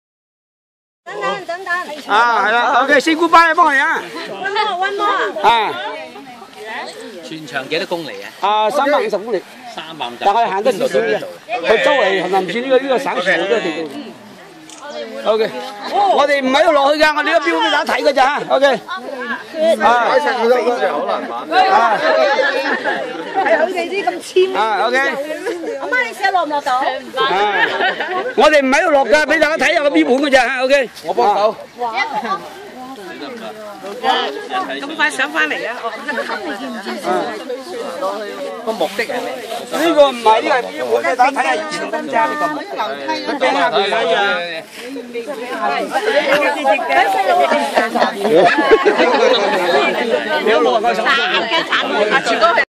Cu Chi Tunnels at Cu Chi Vietnam (S) Mon 3rd Feb 2014.
Cu Chi Tunnels at Cu Chi town in South Vietnam Mon 3rd Feb 2014. The tunnels is 200km long built during the Vietnam war.